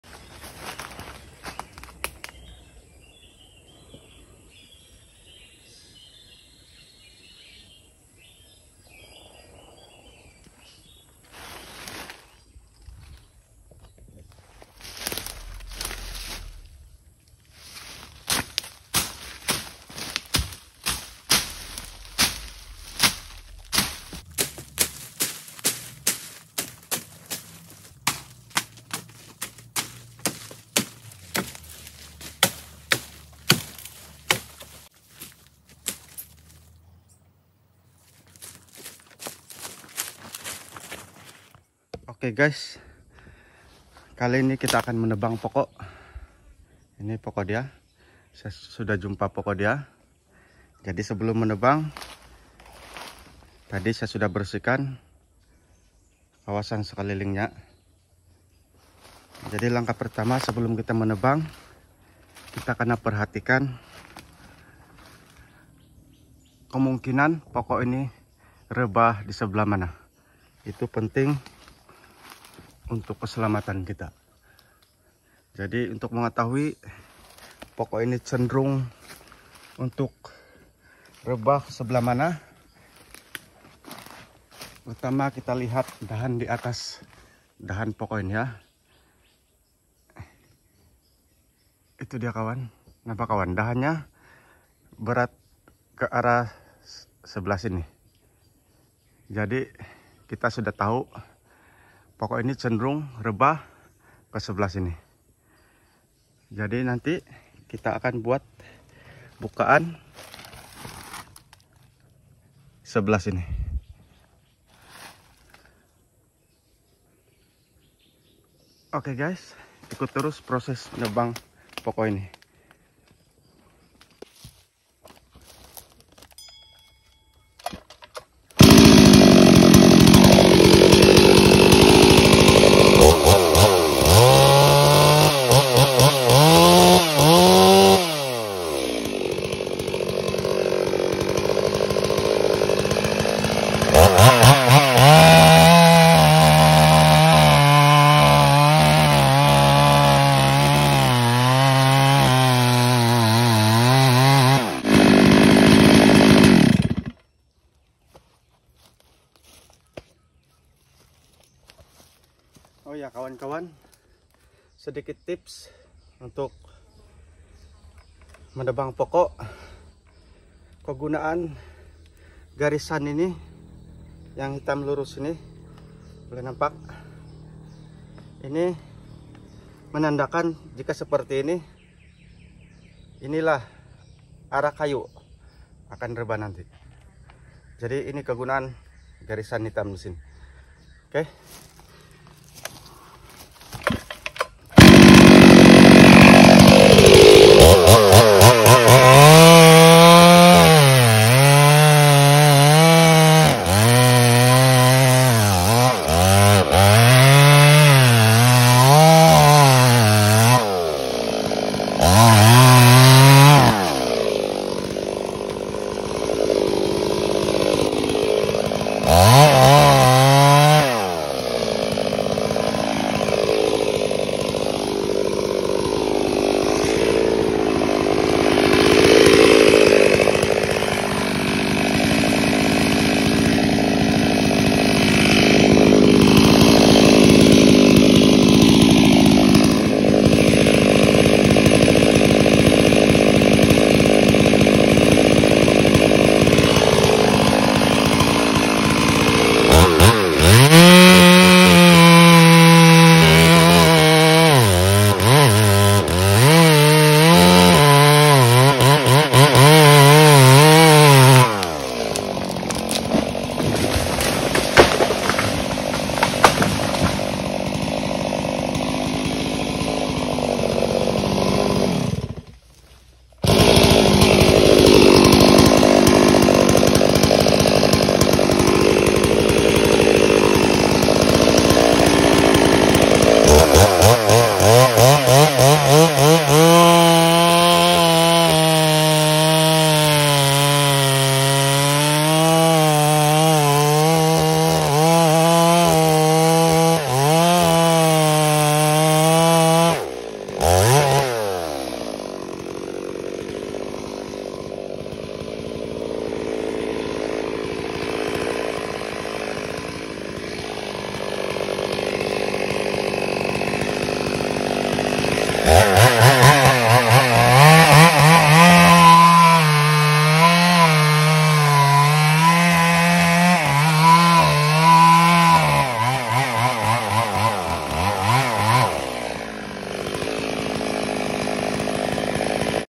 Cara tebang pokok yang betul dengan chainsaw#chainsaw stihl#ms382
Cara atau tutorial menebang pokok yang betul menggunakan chainsaw stihl. Juga sedikit tips yang perlu diketahui sebelum menebang pokok
# chainsaw stihl ms 382
panjang bar= 25”
Rantai oregon
# pokok Akasia